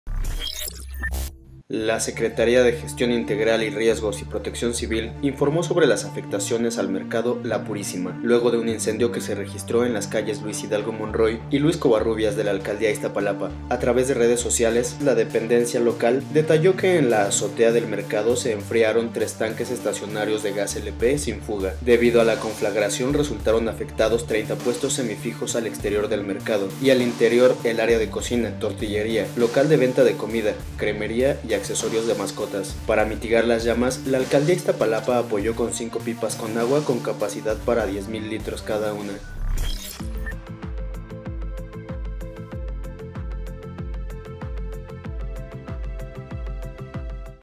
Incendio consume 30 puestos semifijos al exterior del mercado de "La Purísima" en Iztapalapa
Al exterior del centro de abasto resultaron afectados 30 puestos semifijos; y al interior, el área de cocina, tortillería, local de venta de comida, cremería y accesorios de mascotas